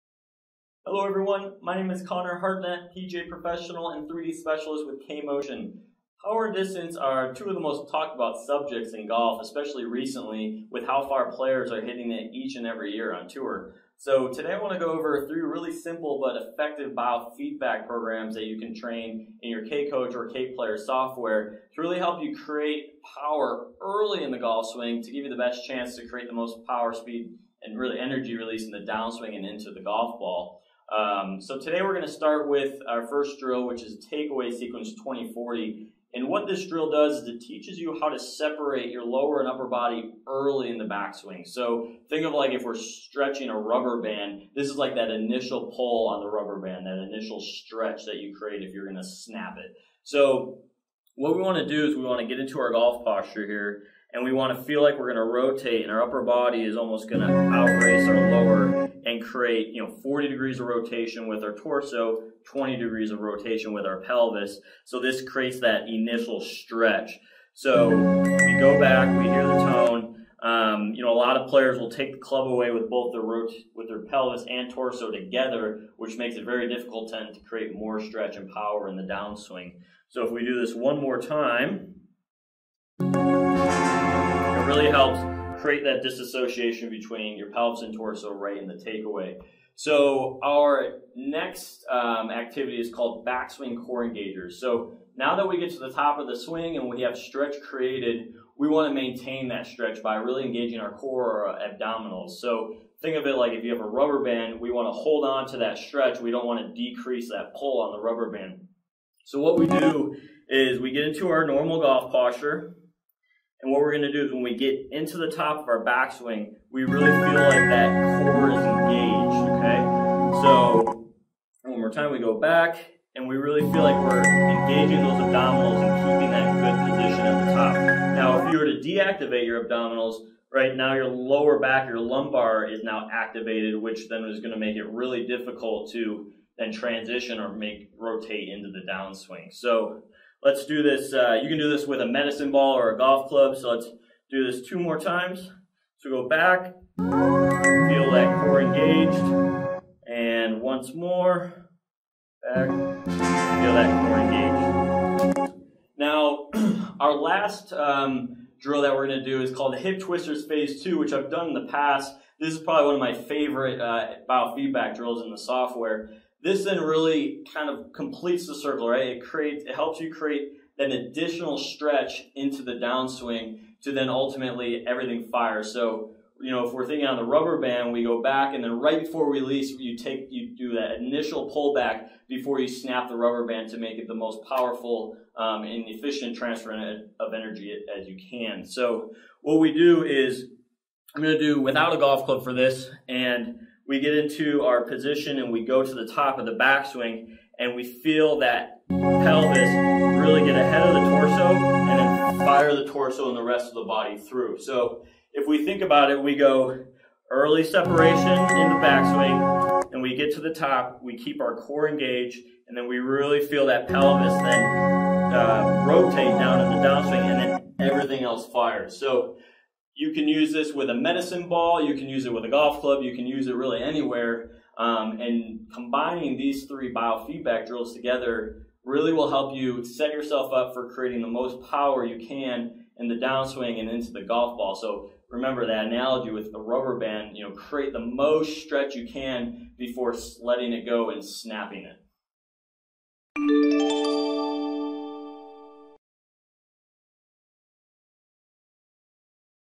Create More Power in your Golf Swing with K-VEST
Creating the right amount of stretch in the backswing is key to providing an athlete the opportunity to use it and generate explosiveness in their downswing.